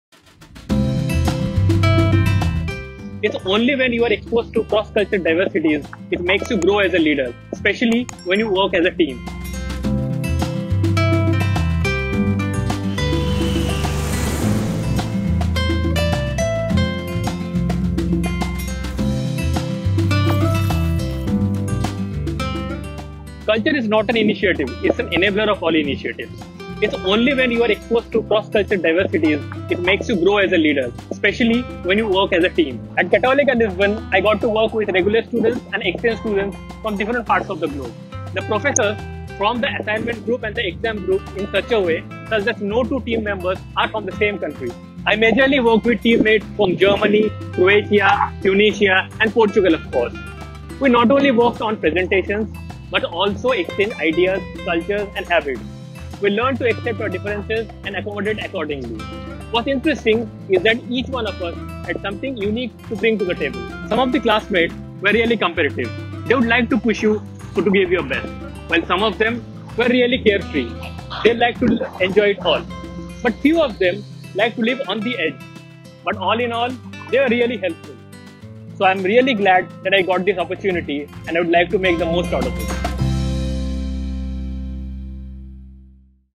From Goa to Lisbon for the Student Exchange Program -Episode 1
"We learn to accept our differences and accommodate accordingly.", says Mr Amir Hossain, PGDM 2020-22 batch, while crediting his professional growth to working with cross-cultural teams.
Watch the video to discover more about his erudition from IEP in Portugal at Católica Lisbon School of Business & Economics.